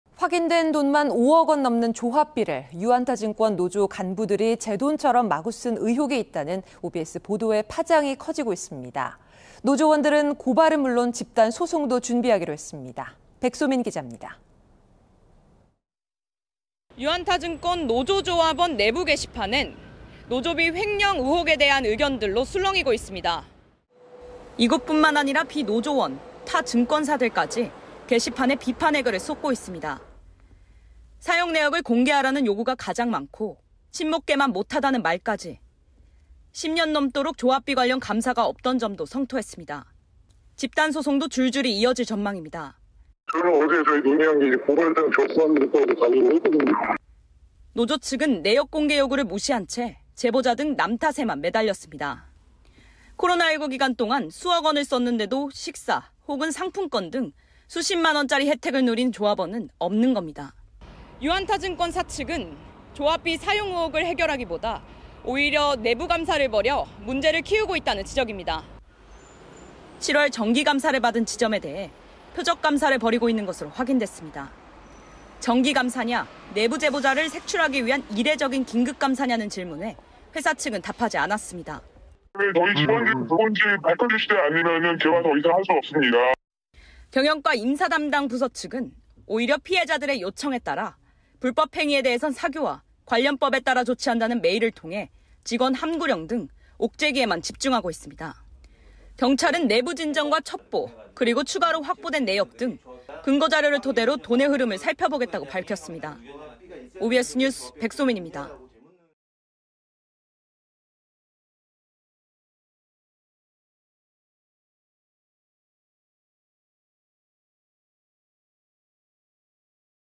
5억 원 이상 조합비 횡령 의혹... 집단소송 준비하는 노조원들
확인된 돈만 5억 원 넘는
조합비를
유안타증권 노조 간부들이
제 돈처럼 마구 쓴 의혹이
있다는 OBS 보도의 파장이
커지고 있습니다.
노조원들은 고발은 물론
집단소송도 준비하기로 했습니다.
백소민 기자입니다.